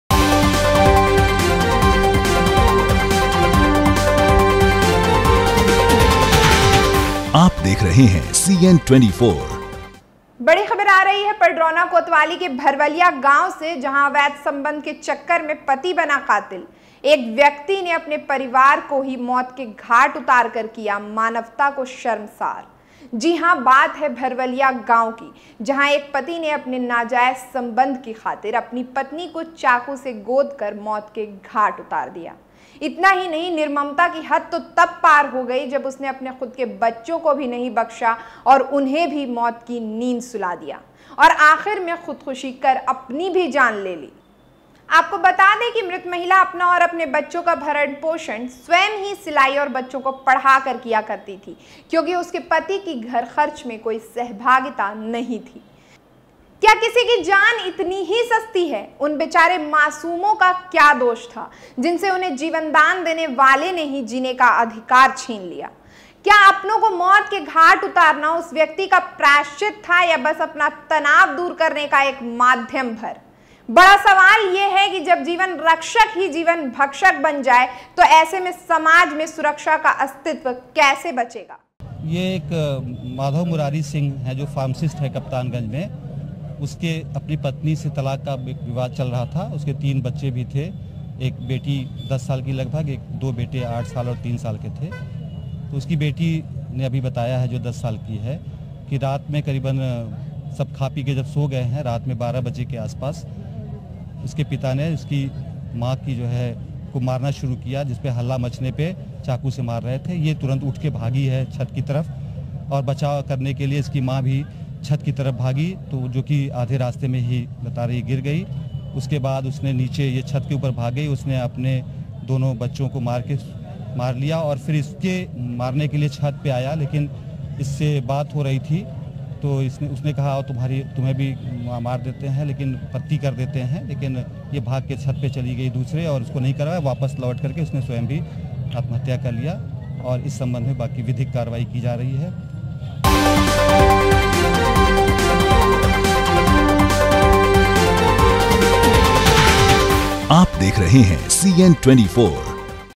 breaking - पति ने की पत्नी समेत बच्चो की हत्या
एक व्यक्ति ने अपने ही परिवार को मौत के घाट उतार कर किया मानवता को शर्मशार

जी हां  बात है भरवलिया गाँव की जहां एक पति ने अपने  नाजायज़ सम्बन्ध की खातिर अपनी  पत्नी को  चाक़ू से गोद कर मौत के घाट उतार दिया| इतना ही  नहीं निर्ममता की हद्द तो तब पार हो गई  जब उसने अपने खुद के बच्चों को भी नहीं बक्शा और उन्हें भी  मौत की नींद सुला दिया

और आखिर में खुदखुशी कर अपनी भी  जान ले ली,

 आपको  बता दे की मृत महिला अपना और अपने बच्चों का भरण पोषण स्वयं ही सिलाई और बच्चों को पढ़ाकर किया करती थी क्योंकि उसके पति की घर खर्च में कोई सहभागिता नहीं थी